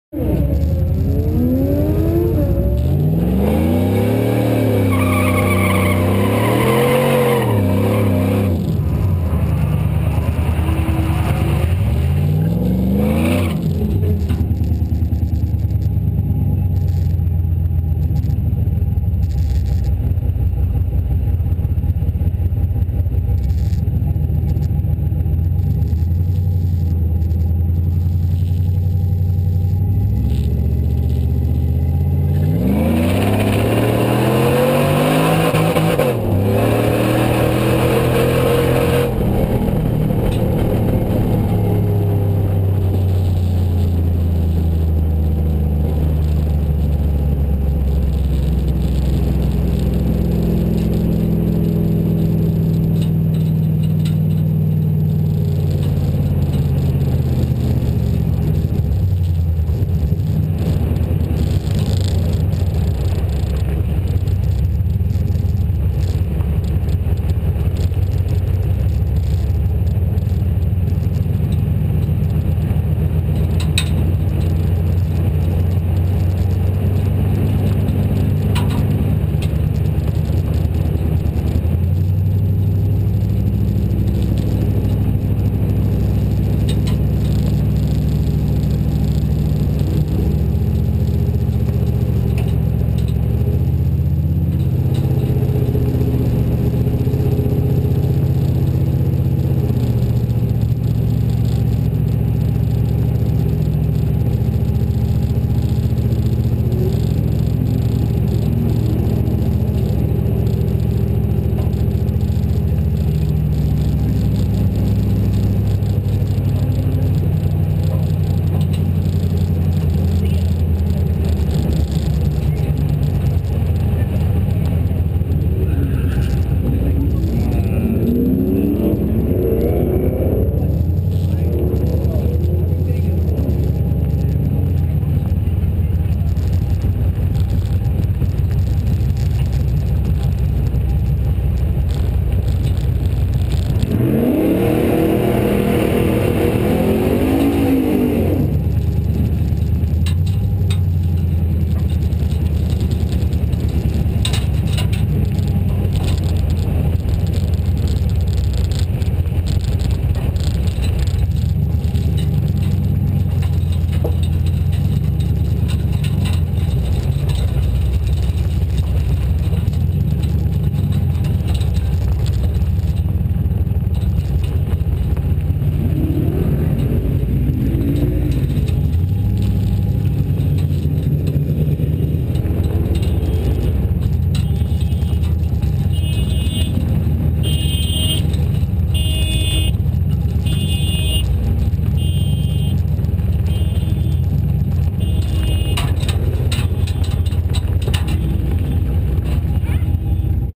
hugo slipping tranny
buddies Bad run tranny wouldnt shift